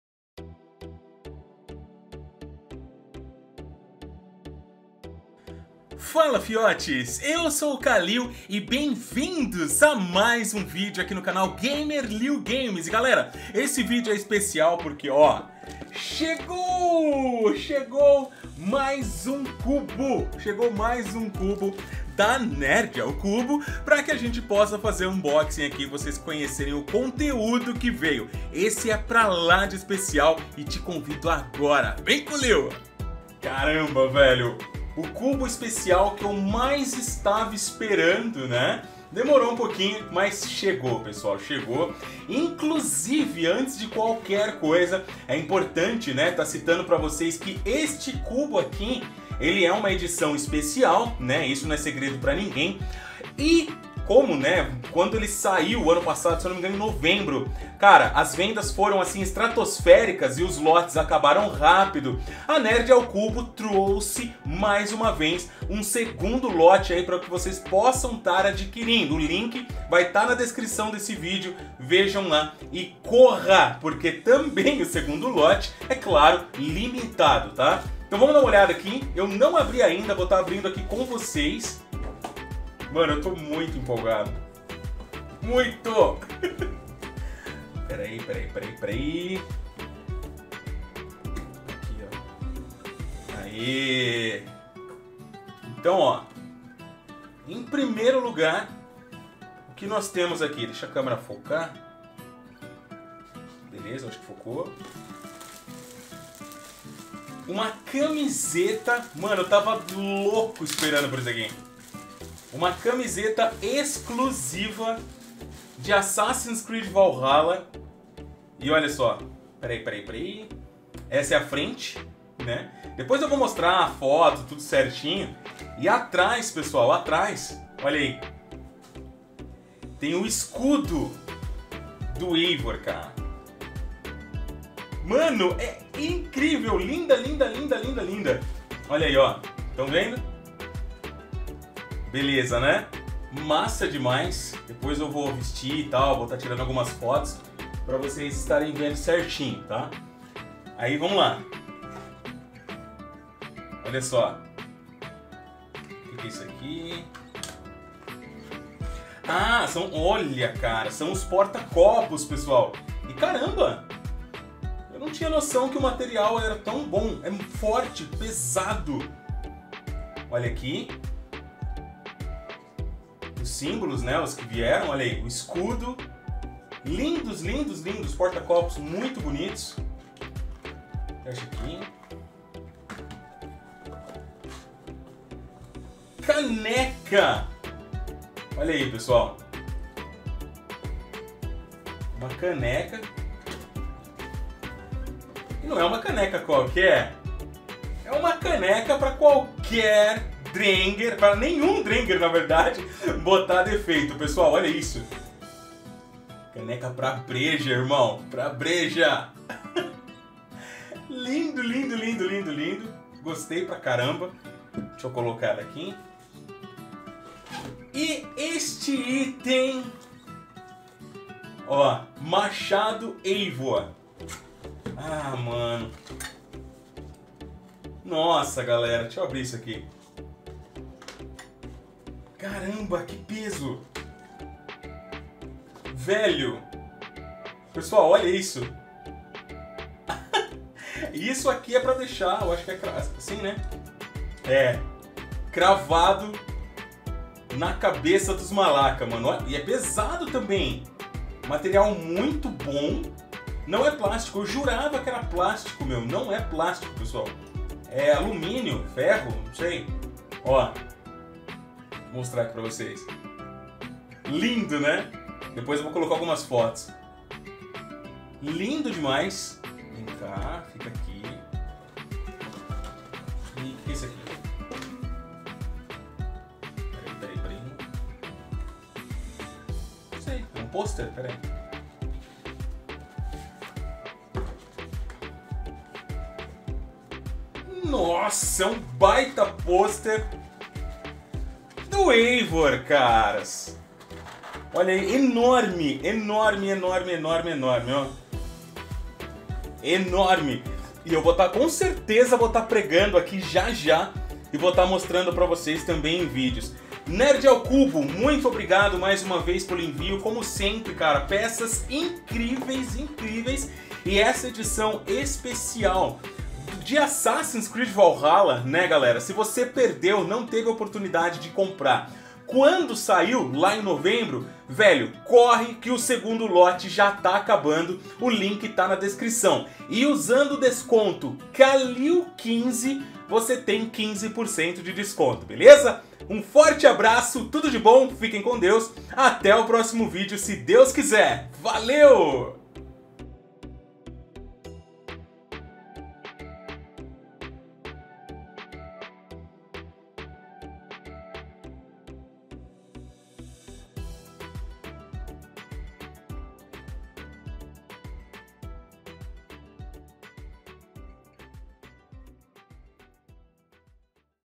Edição Especial Assassin's Creed Valhalla da @NerdaoCubo [ Unboxing 4K ]
Fala Fiotes!
Como estão?
Chegou meu tão sonhado Cubo especial da @NerdaoCubo que trouxe muitos itens exclusivos inspirados em Assassin's Creed Valhalla!
Um novo lote está disponível e se você ainda não encomendou o seu, corra agora que é limitadíssimo!
Usando o cupom "Kallil15" ganhe descontão!

Kallil Augusto